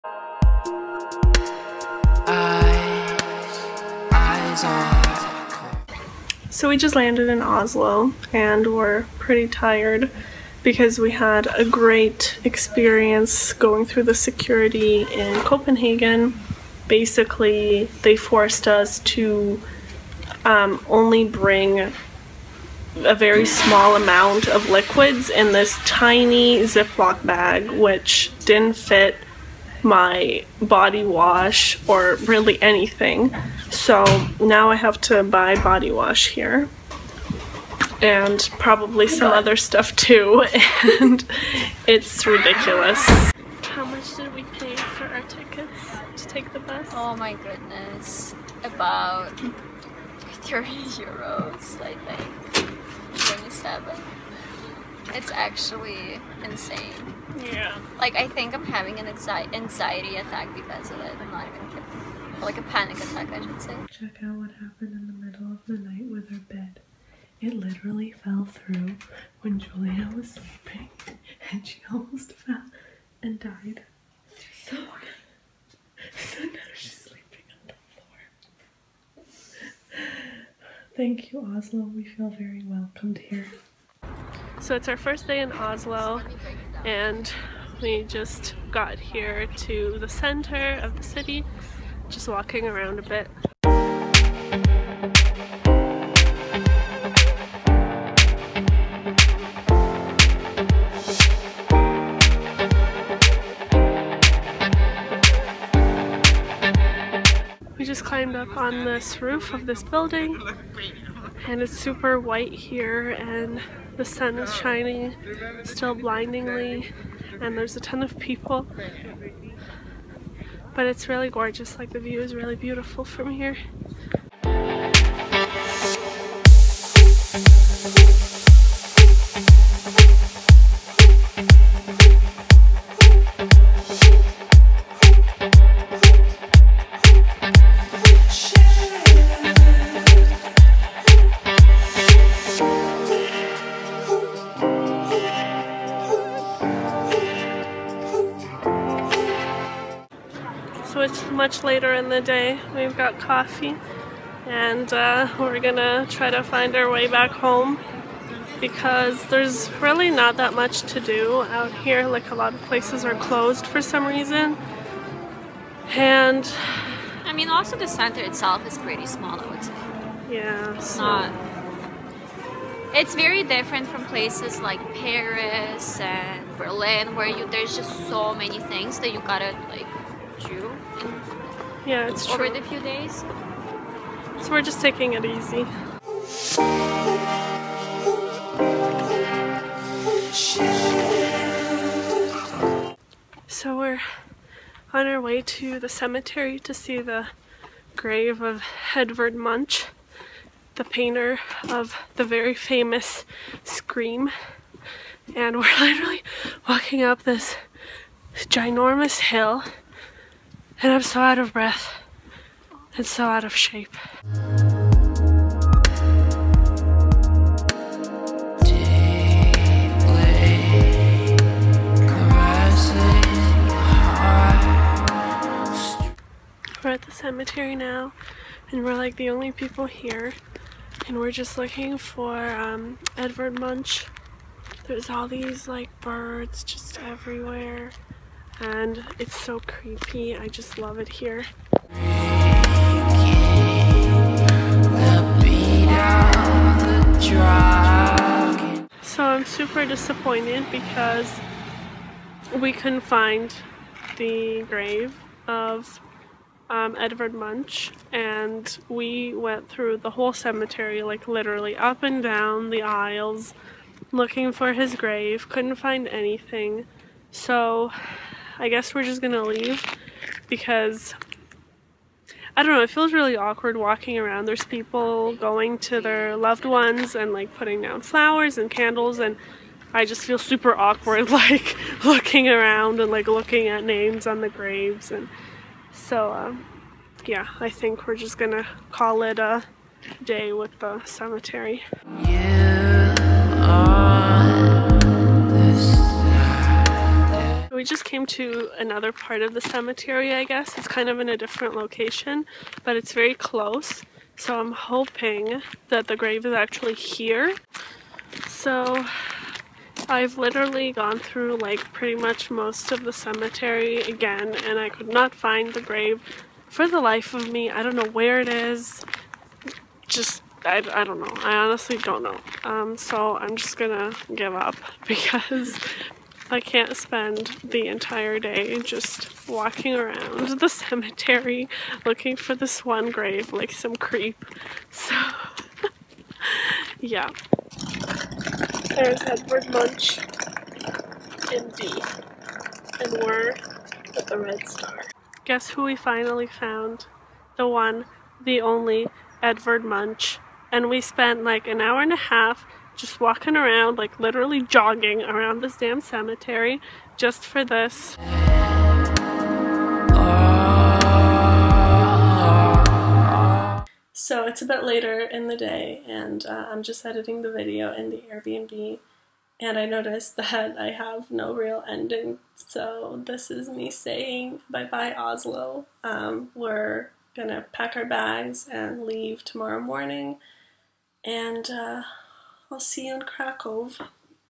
Oslo Vlog // Quest for Edvard Munch
We spent two days in Oslo, enjoying the city despite the cold.